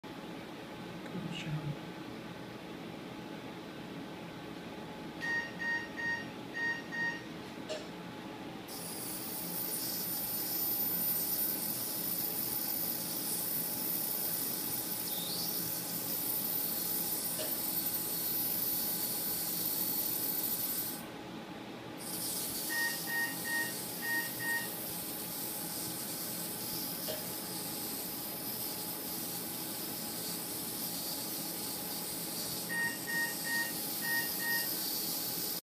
Dog patient undergoing cataract surgery at CullenWebb Animal Eye Specialists
Brief clip of a dog undergoing cataract surgery at CullenWebb Animal Eye Specialists